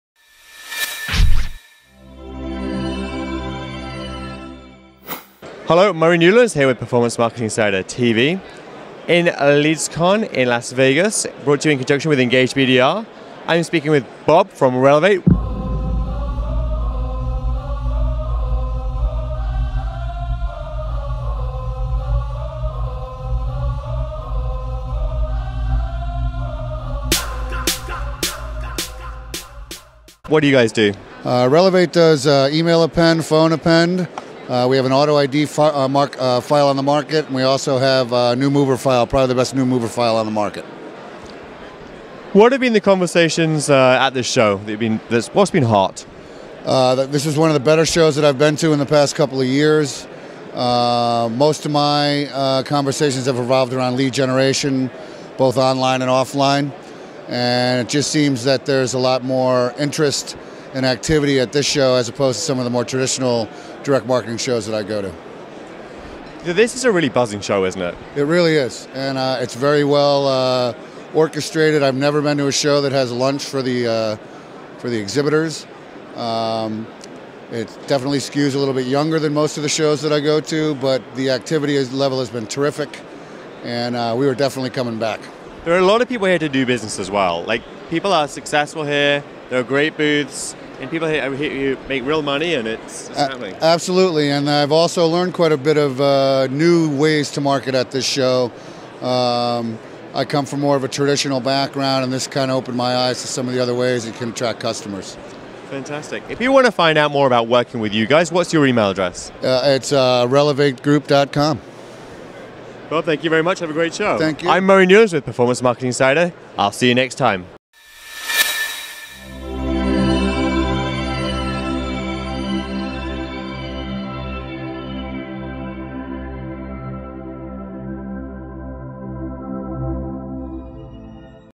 New Ways to Market with Bob Lilley of Relevate at LeadsCon 2013 in Las Vegas
please. Bob Lilley of Relevate discusses lead generation and traditional marketing with Murray Newlands at LeadCon 2013 in Las Vegas